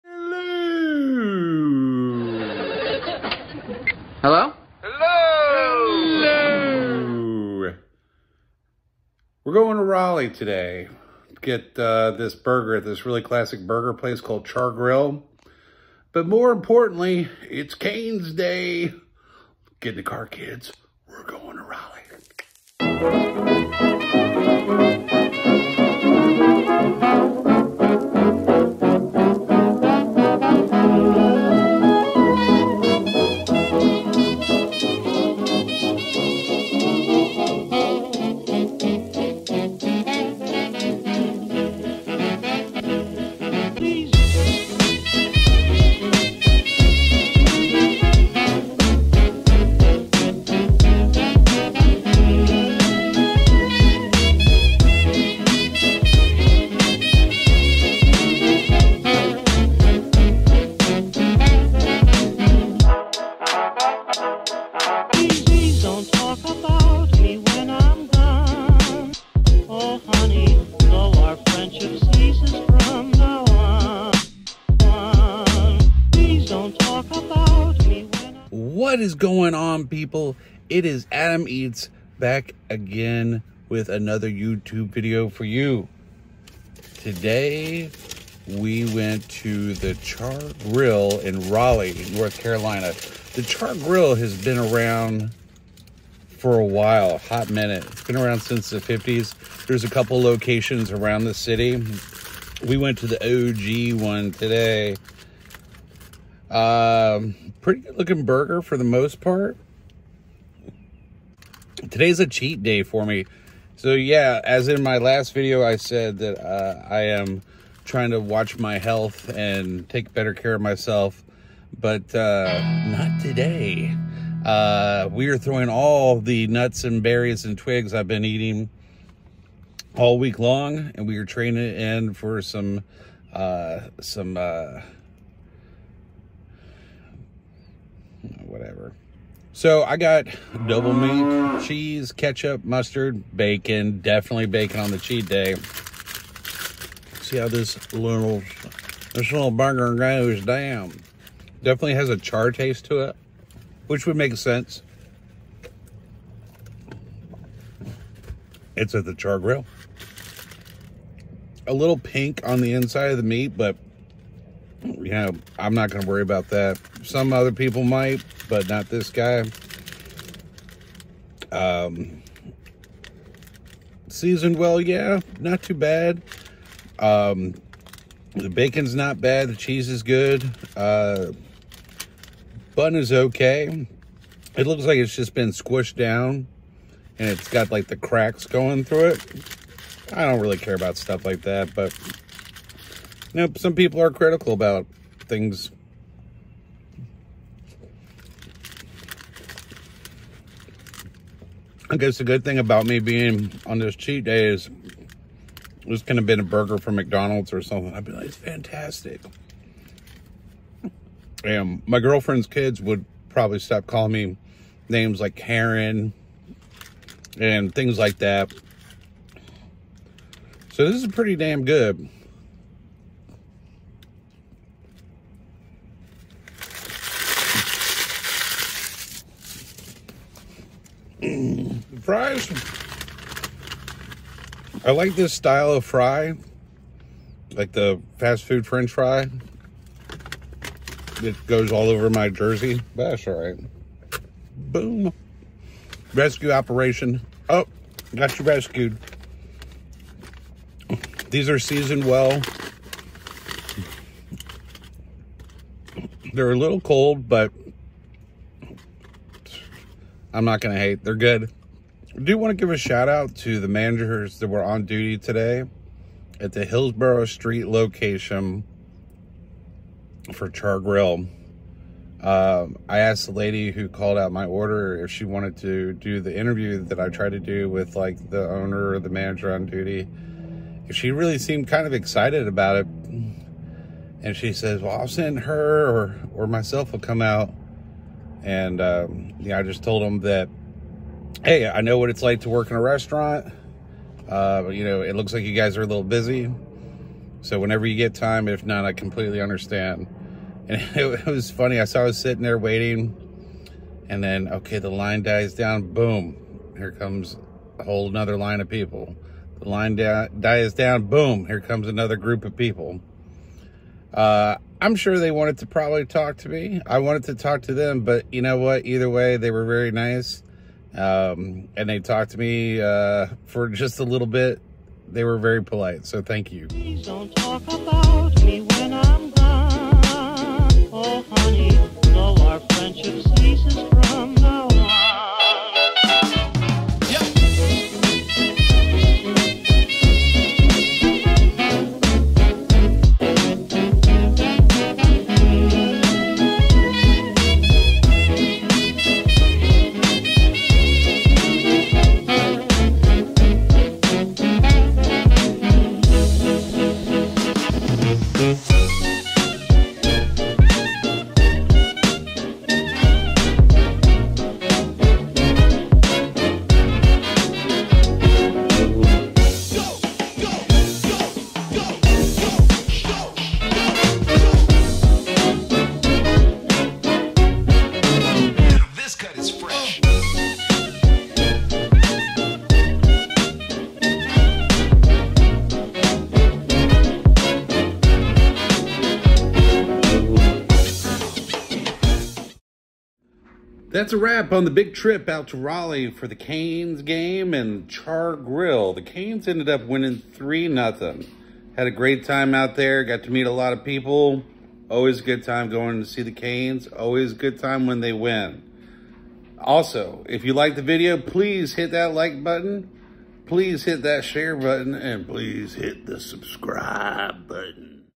Char-Grill: Double Cheeseburger w/ Canes game
I spent the day traveling to one of Raleigh’s oldest restaurants. The Char-Grill has several locations throughout the Raleigh area, but we went to the original location today. The weather was perfect for a delicious cheat day and even more perfect for a tailgate at the Carolina Hurricanes hockey game.